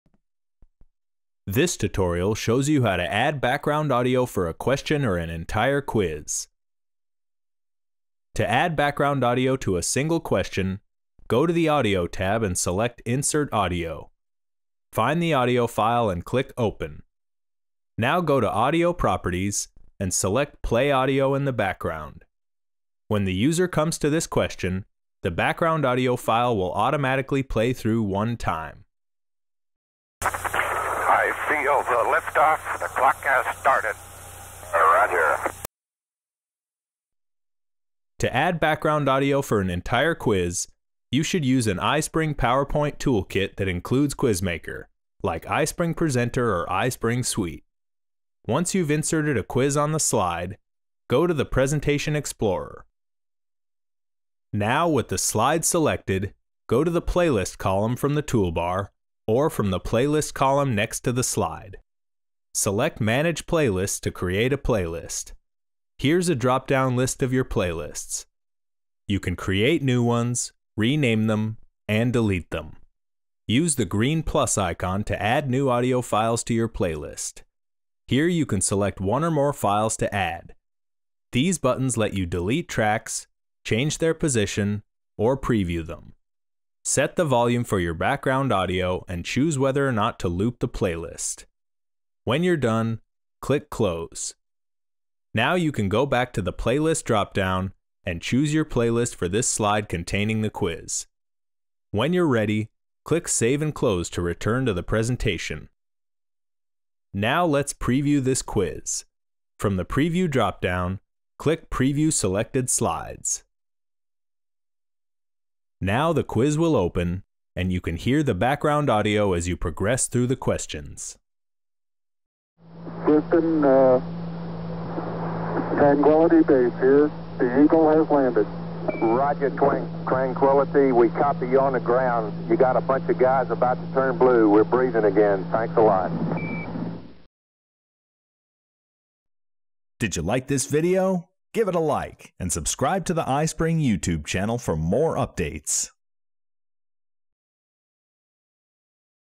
How to Add Background Audio for a Question or an Entire Quiz
This tutorial shows you how to add background audio for a question or an entire online quiz.